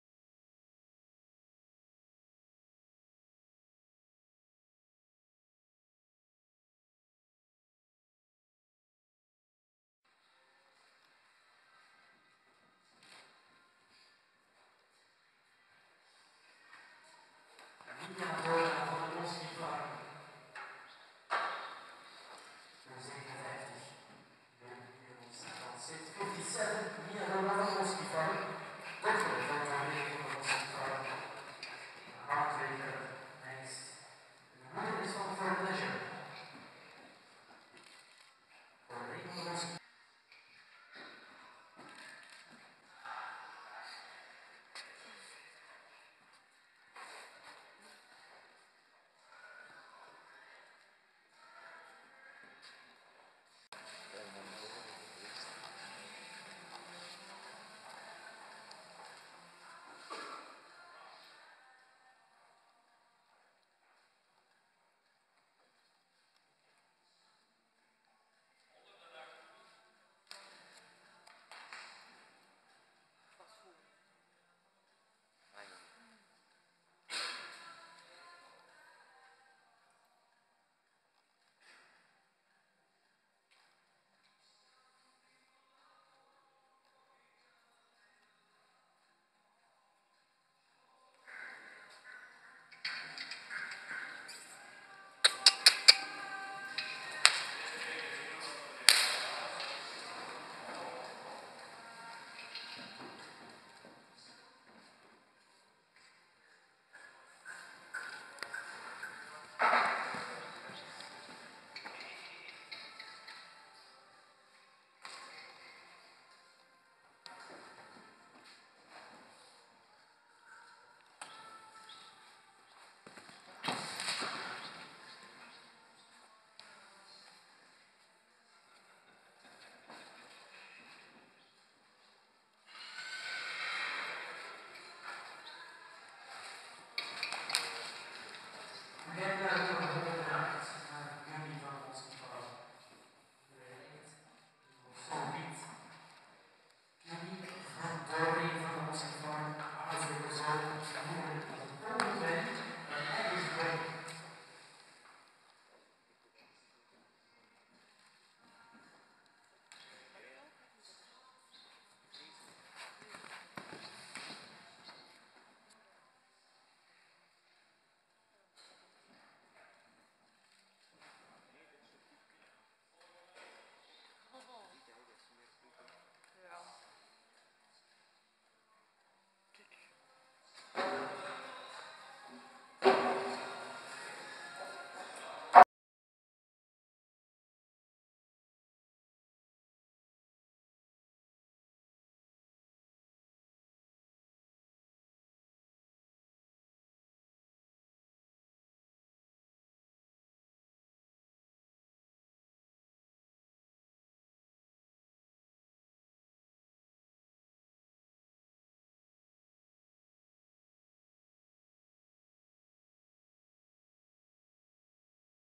2014-12-06 Mia en Gummy van de Moskifarm Qualified Jumping Mechelen
Mia van de Moskifarm (Filly 2 years) & Gummy van de Moskifarm (Stallion 2 years) qualified for the Great Belgian National Loose Jumping  for two years old youngsters show jumpers at the Christmas Jumping in Mechelen on 27/12/2014  09:99am